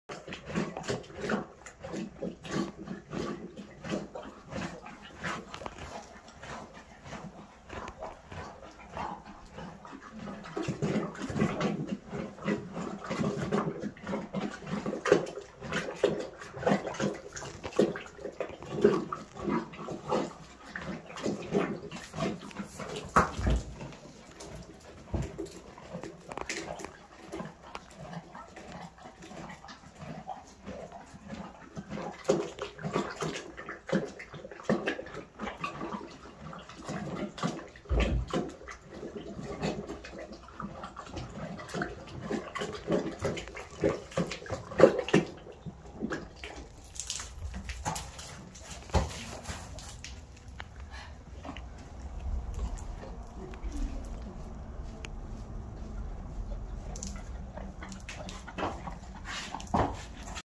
ziva en zijn slobber.
Slobber, paarden houden van die natte smurrie die goed voor hun darmen schijnt te zijn.